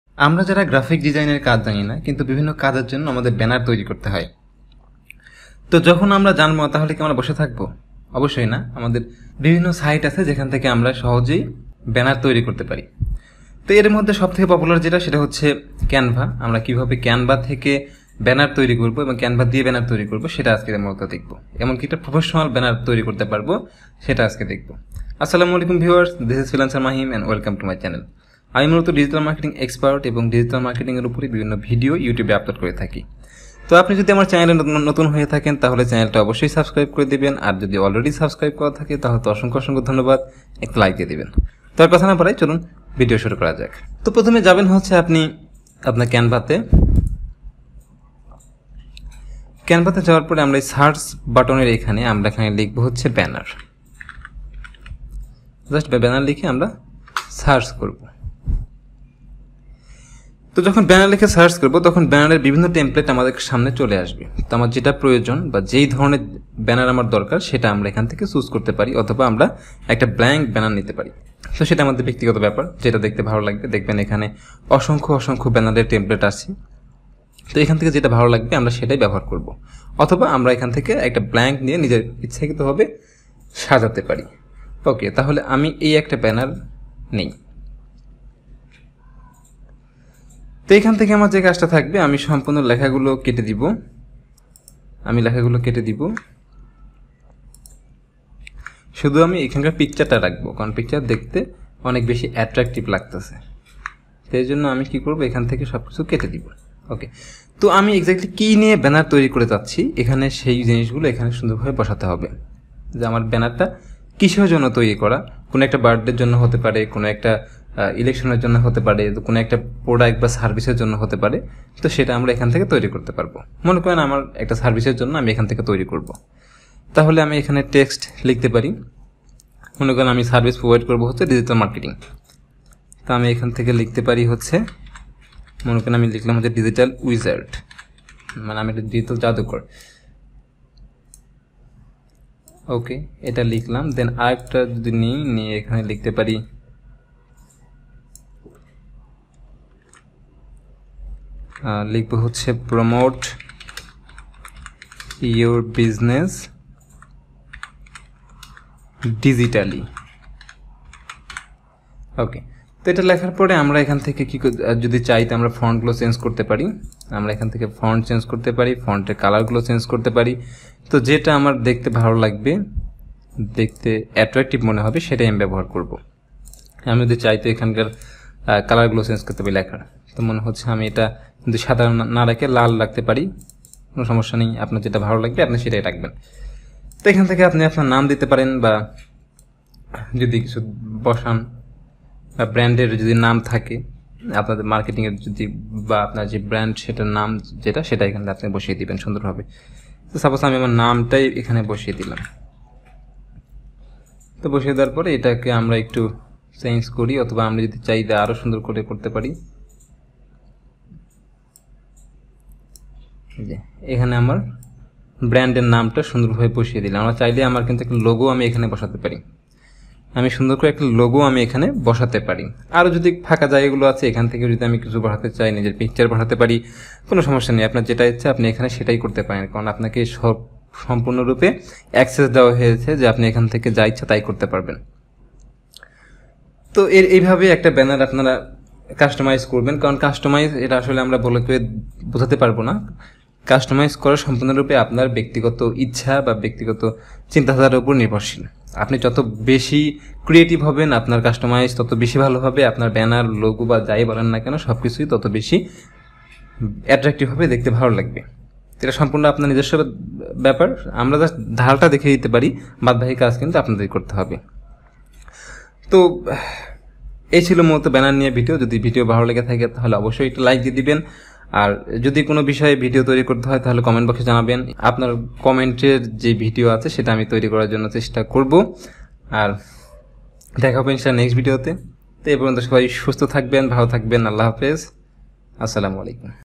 Professional Banner Create With Canva | Freelancer Mahim
In this video i will share with you how to create a proffessional banner with canva.